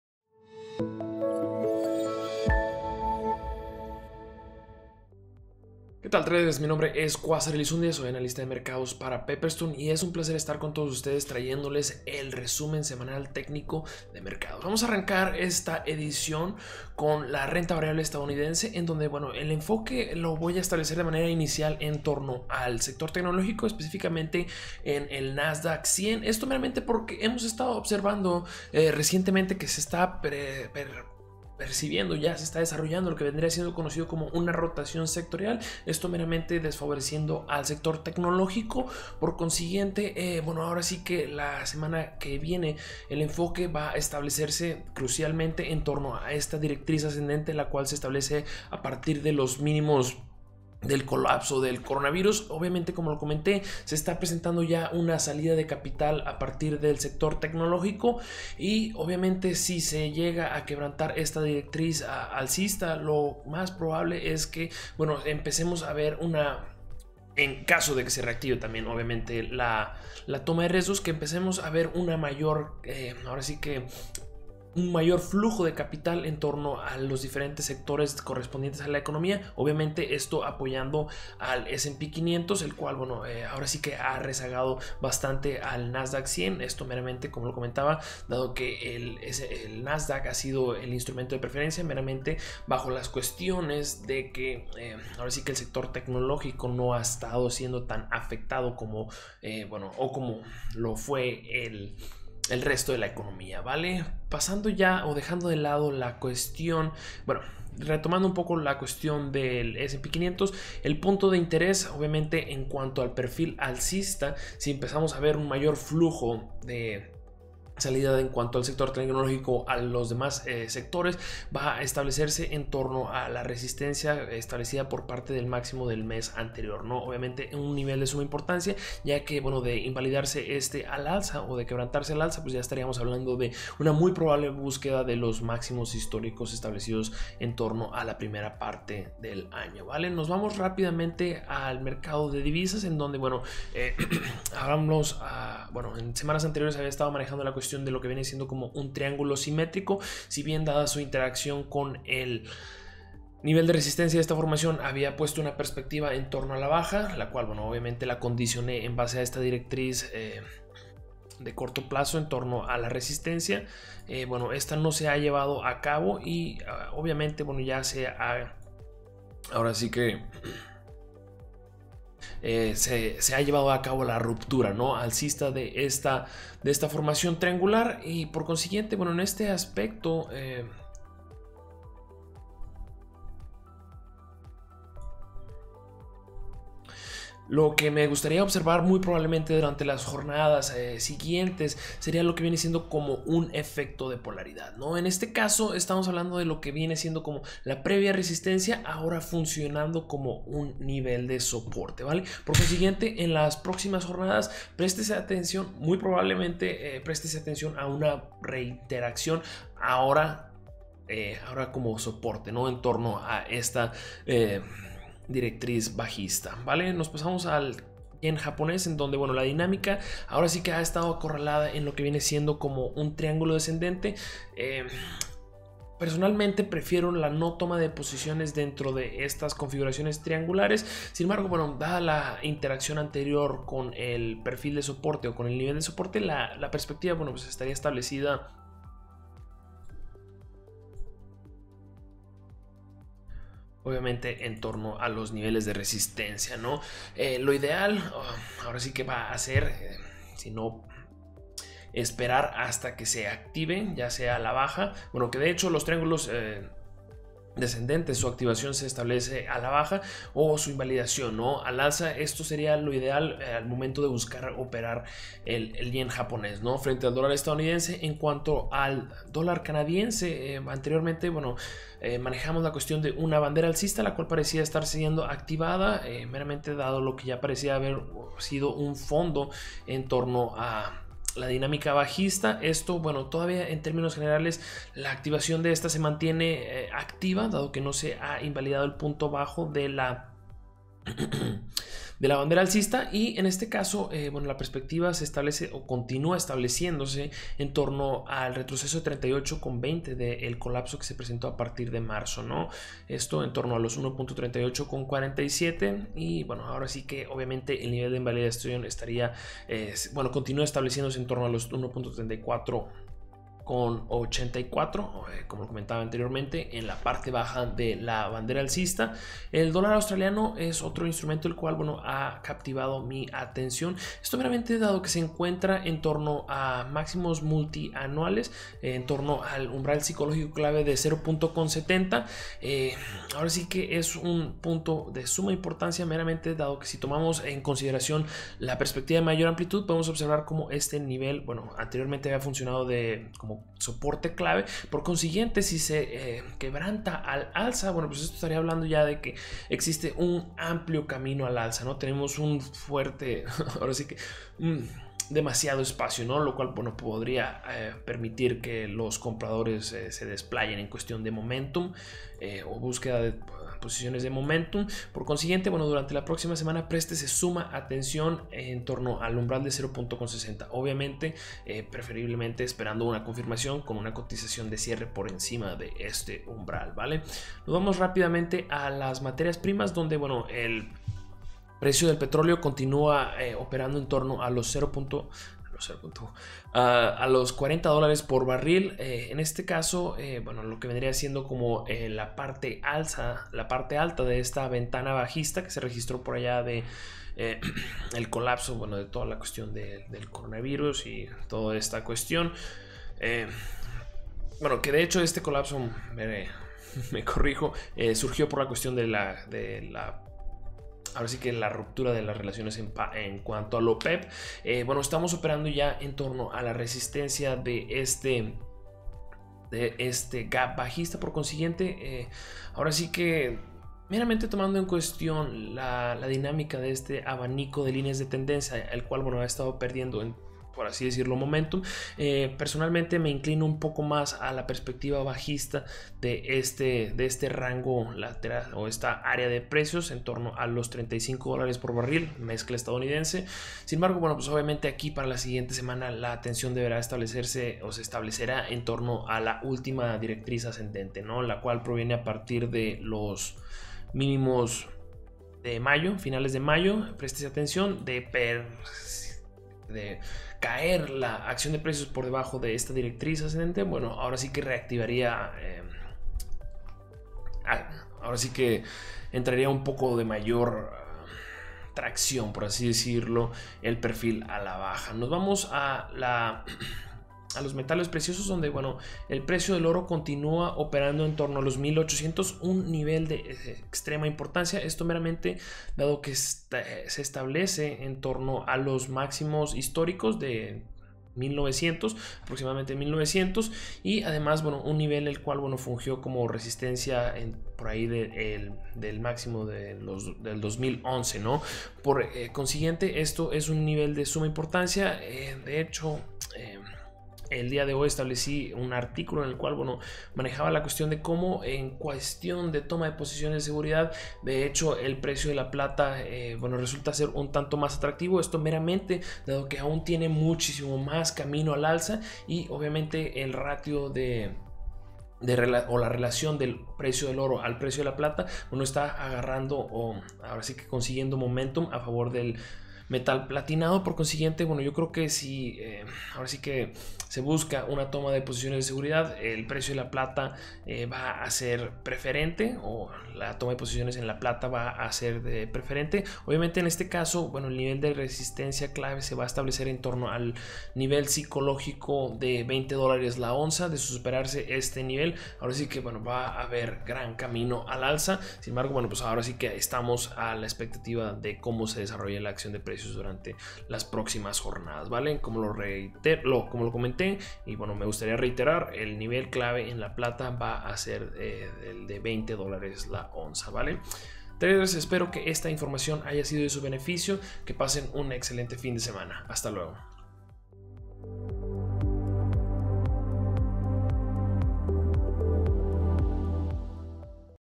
Análisis técnico de mercados: 17 de julio de 2020
En este video Quasar Elizundia del equipo de Pepperstone nos explica las principales configuraciones técnicas en los mercados.